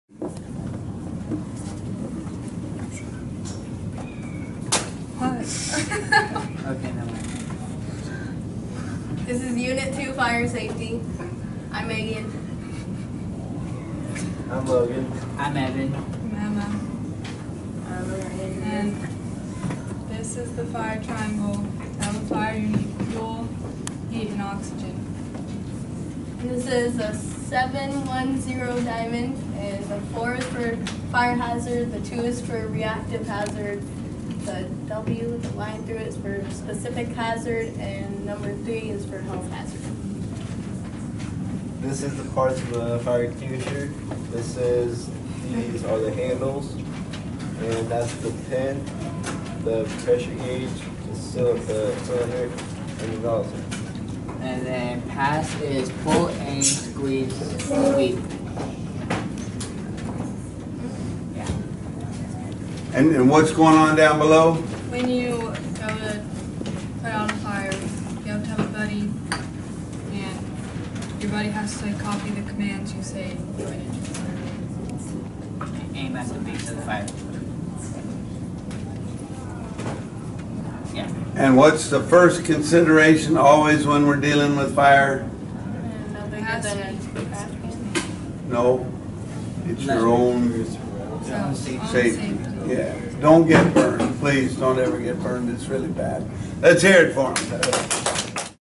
Taft Teen CERT Presentation of Unit 2, Fire Safety
Taft Teen CERT 1st Semester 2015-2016 Presentations